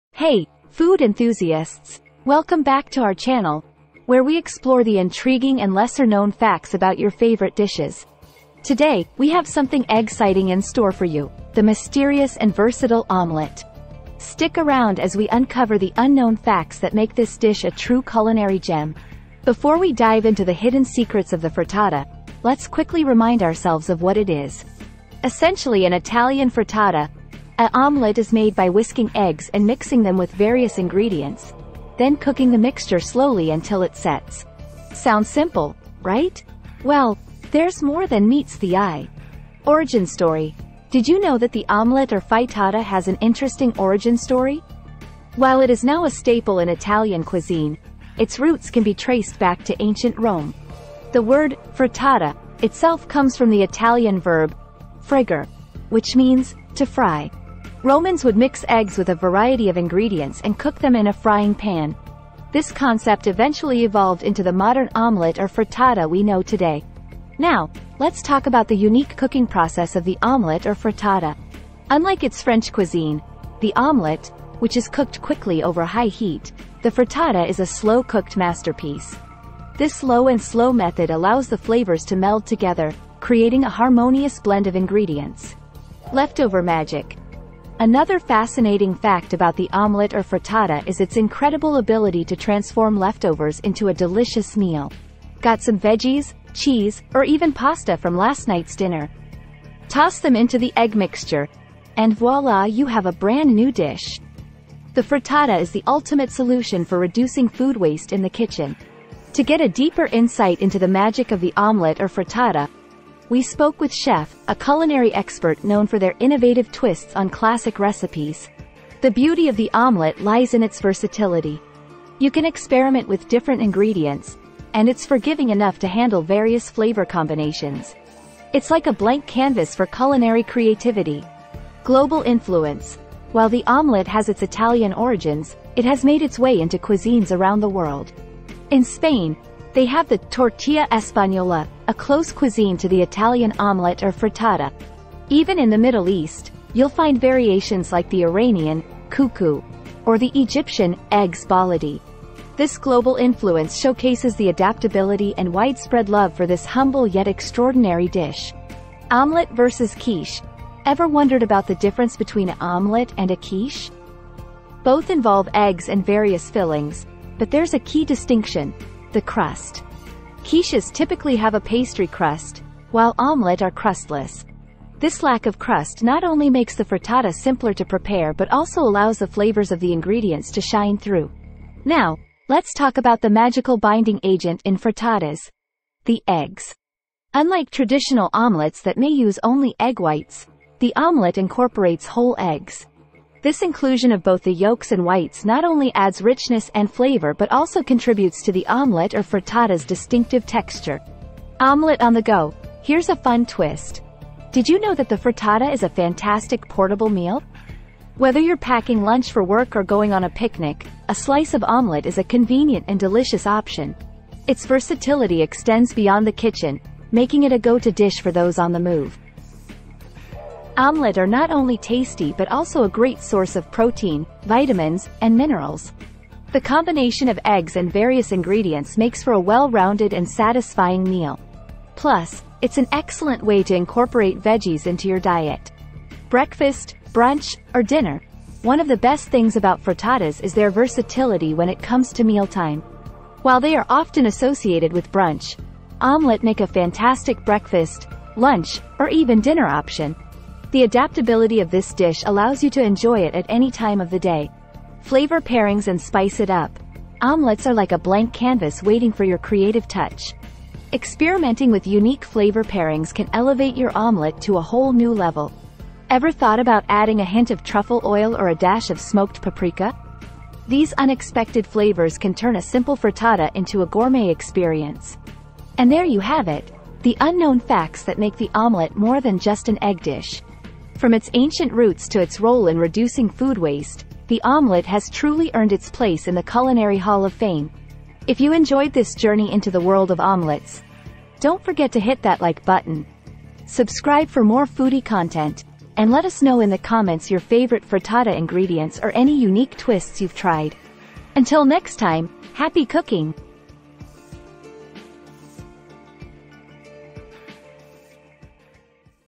Unveiling the Secrets of Italian omelette
"Unveiling the Secrets of Italian Omelette" explores the traditional Italian dish known as the frittata. This versatile and hearty omelette is celebrated for its simplicity and rich flavors. The article delves into its key ingredients, which typically include eggs, vegetables, meats, and cheeses, and emphasizes the importance of fresh, high-quality components. It also highlights the preparation techniques that distinguish a frittata from other omelettes, such as cooking it slowly over low heat and finishing it under the broiler for a perfectly set top. By uncovering these secrets, the article sheds light on the cultural significance of the frittata in Italian cuisine and its role as a beloved dish for any meal of the day.